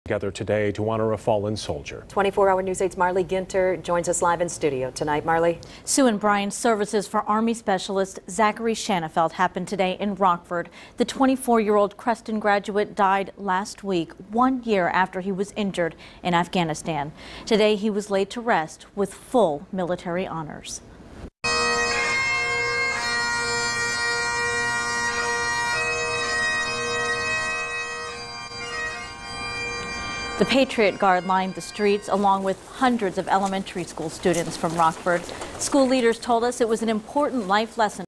Farewell to Army Spc. Zachary Shanafelt
The 24-year-old combat medic was laid to rest Friday,  a week after he died at Walter Reed Army Hospital in DC